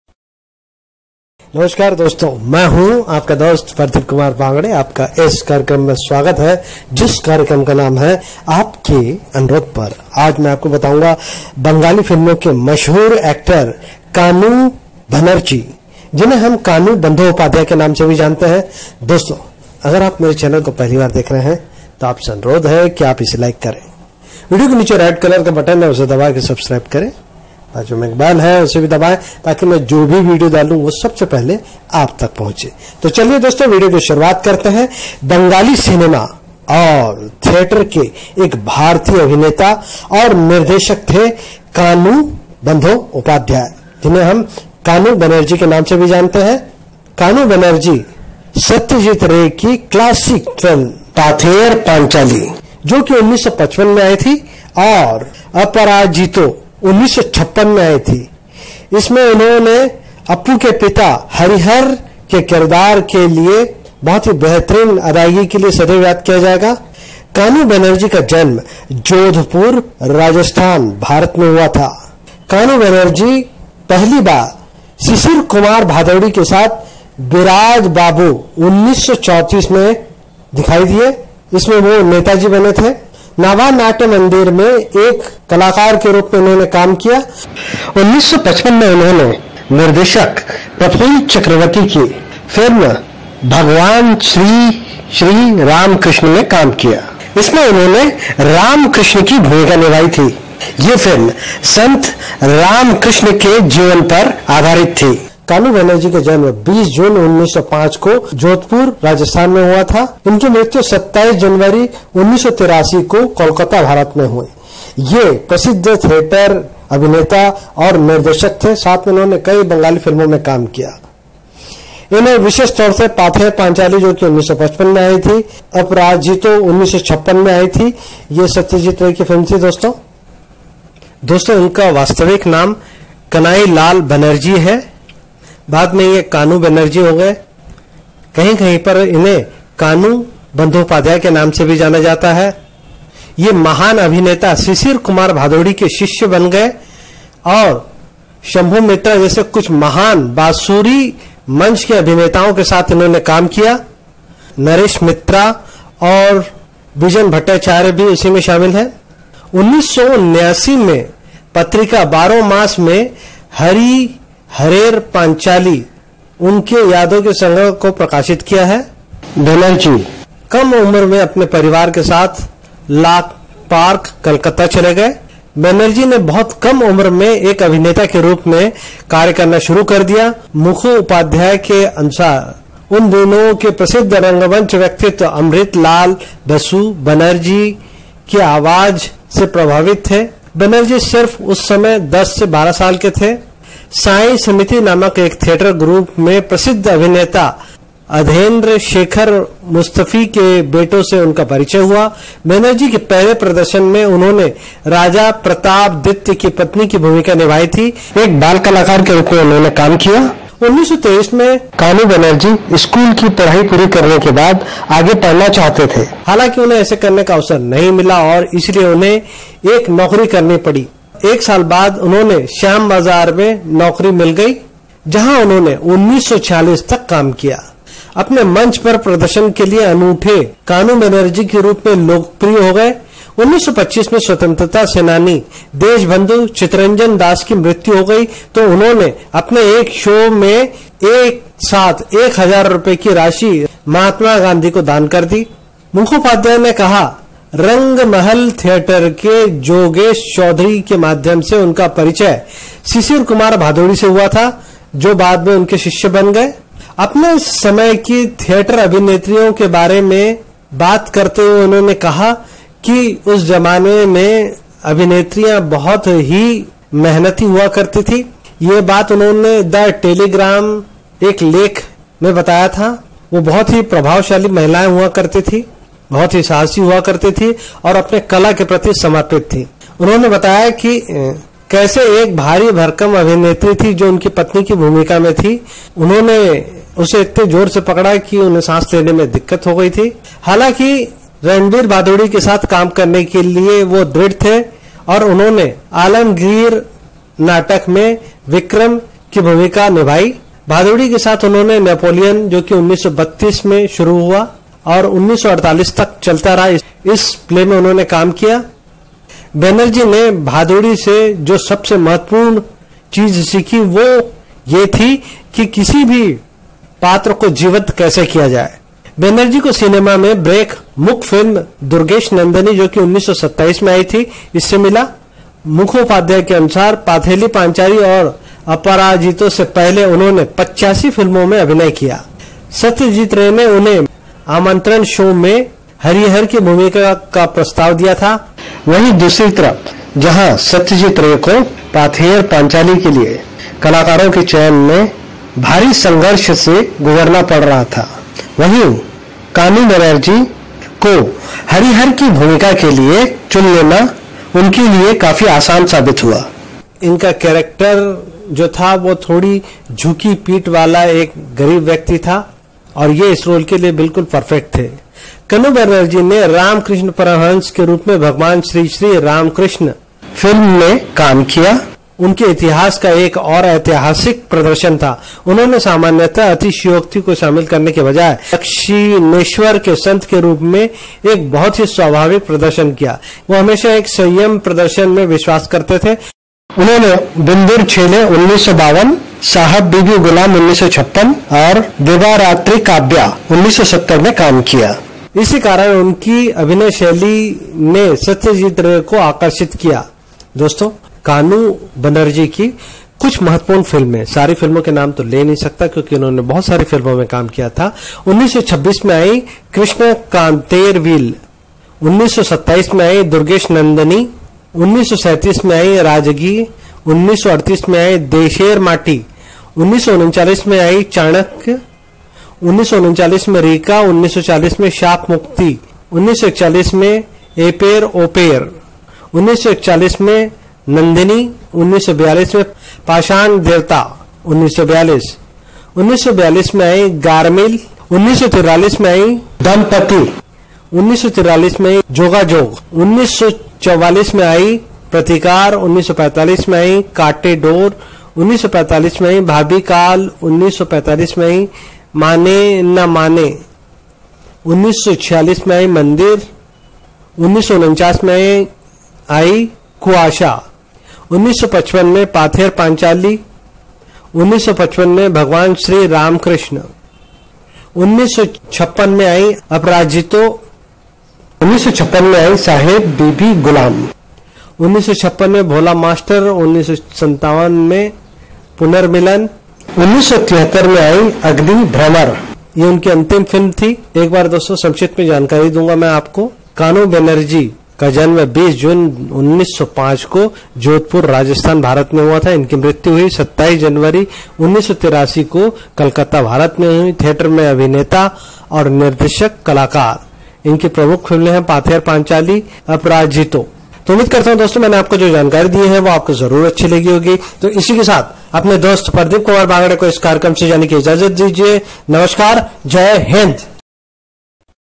कानू बनर्जी प्रसिद्ध अभिनेता - जीवन परिचय
Kanu Banerjee (Kanu Bandyopadhyay) (20 June 1905 – 27 January 1983) was an Indian actor and director of Bengali cinema and theatre. He is best known for his portrayal of Harihar Ray, father of Apu, in Satyajit Ray's classic Pather Panchali (1955) and Aparajito (1956), part of the Apu Trilogy. He was born in Jodhpur, Rajasthan, India. He first appeared as an amateur artiste with Sisir Kumar Bhaduri in Biraj Bou (1934) as Netai at Naba Natyyamandir. In 1955, he also appeared as saint Ramakrishna in Prafulla Chakraborty’s biographical film Bhagaban Sri Sri Ramakrishna.